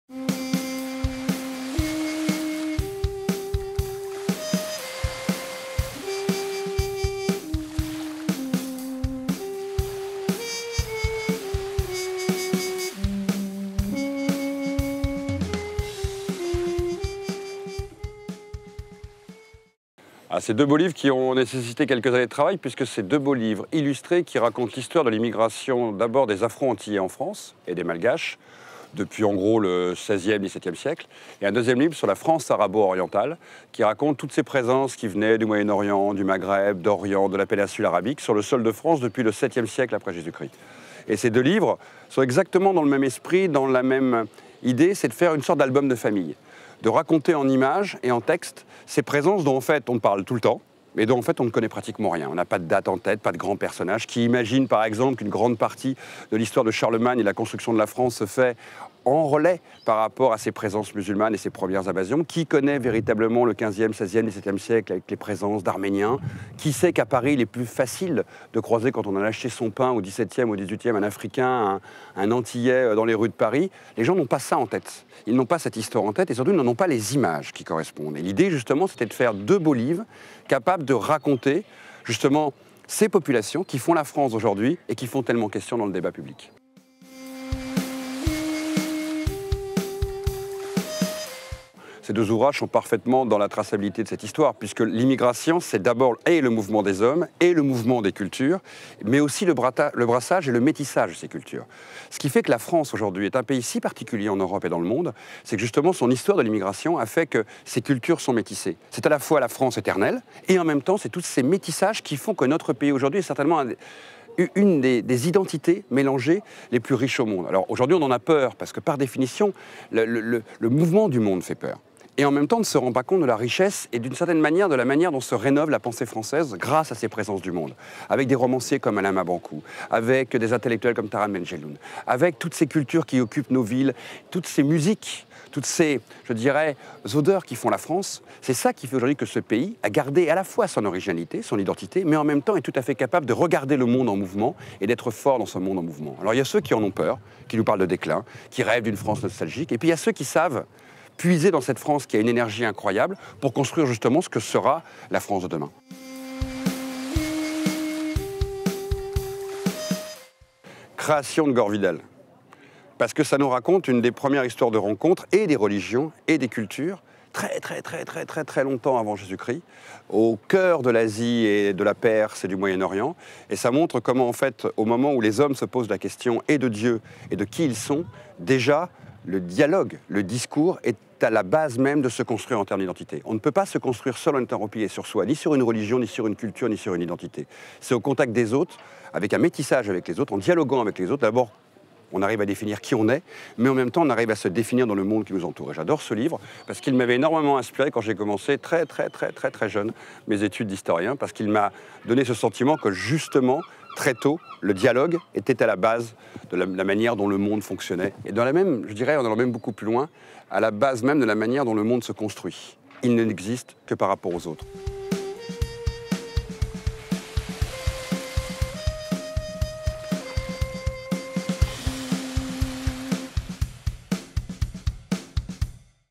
Pascal Blanchard - La France noire
A l'occasion du Festival Etonnants Voyageurs à Saint Malo, rencontre avec Pascal Blanchard autour des ouvrages "La France noire" et "La France arabo-orientale" aux éditions La Découverte.